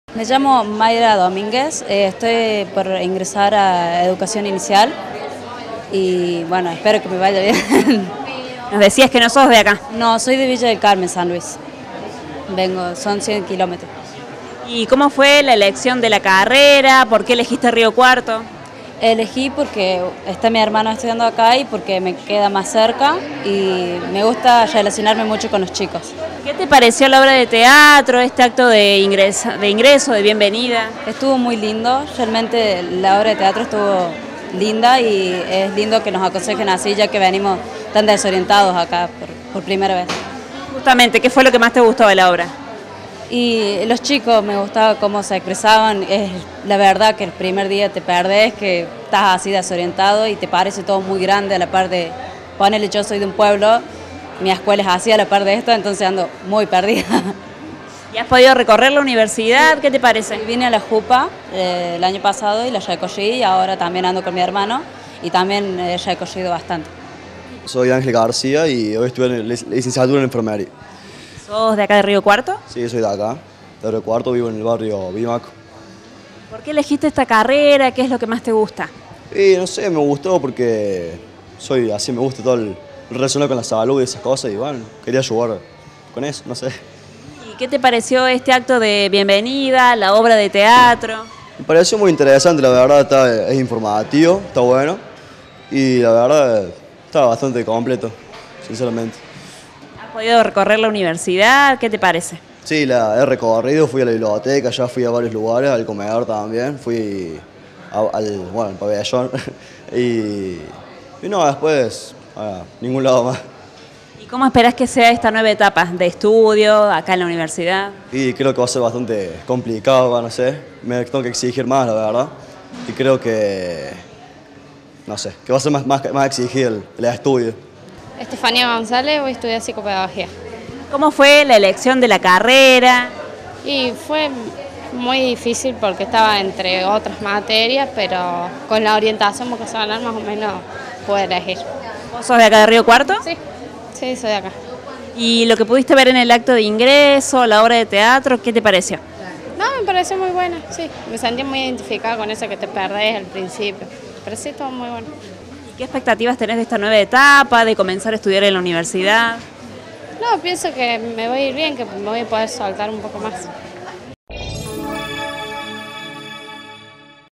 ACTO DE BIENVENIDA
INGRESANTES FACULTAD DE CIENCIAS HUMANAS. LAS EXPECTATIVAS DE LOS NUEVOS ESTUDIANTES QUE COMIENZAN A CURSAR EN LA UNRC
Facebook.com/uniriotv
Twitter.com/uniriotv